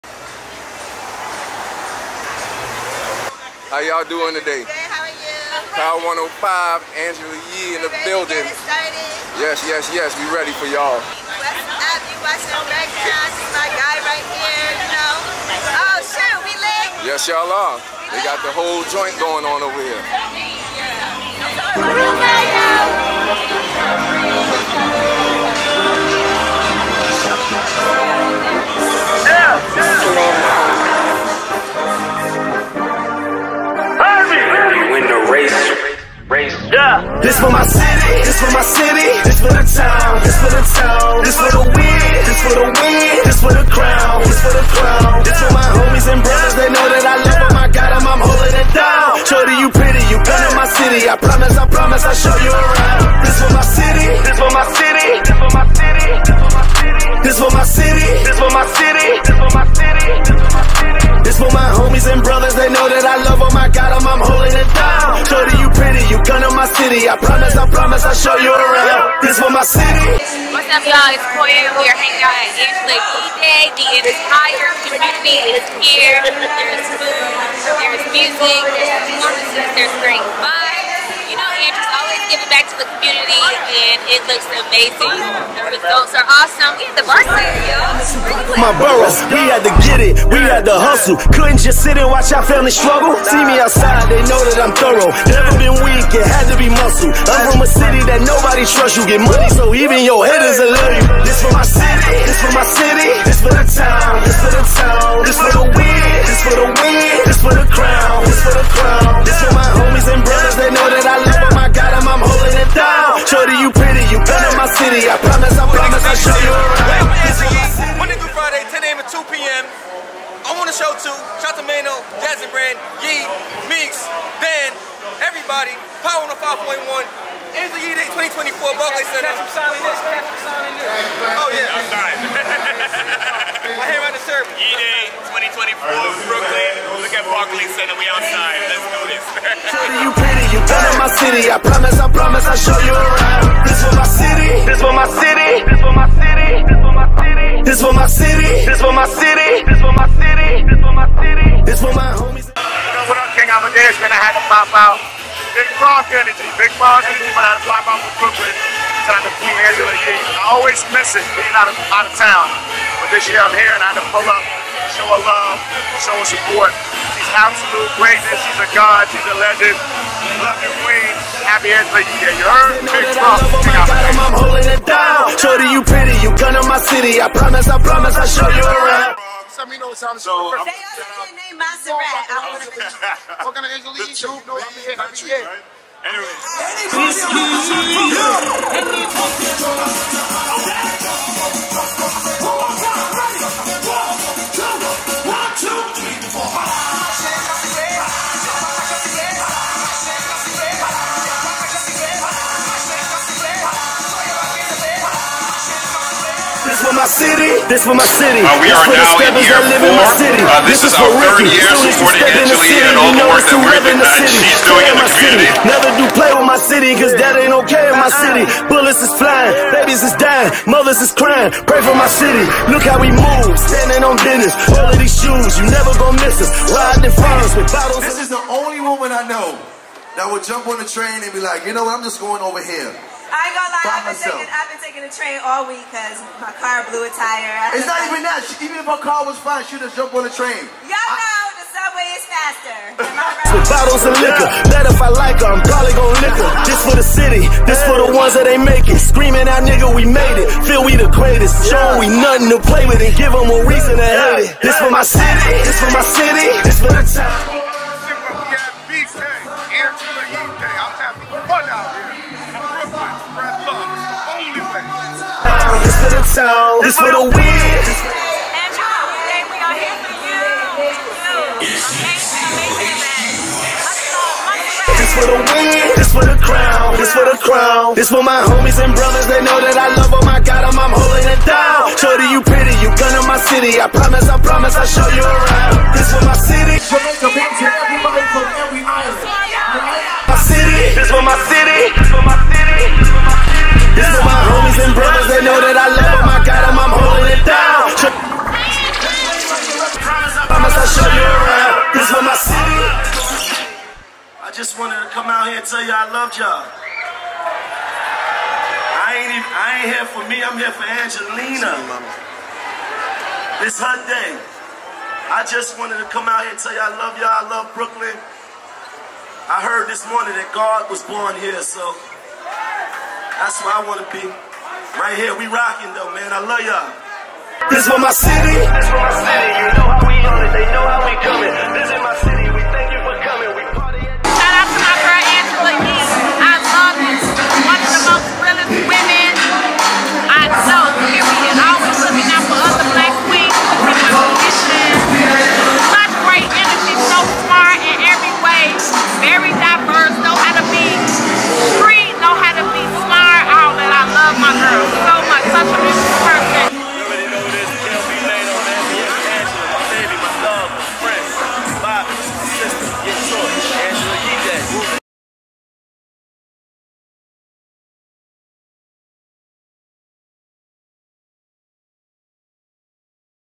ANGELA YEE DAY 2024
@AngelaYee  Power 105 and @wayupwithyee Has a day in Brooklyn and it's called Angela Yes Day.  This featured some great performers and vendors. People came from all over to celebrate a wonderful day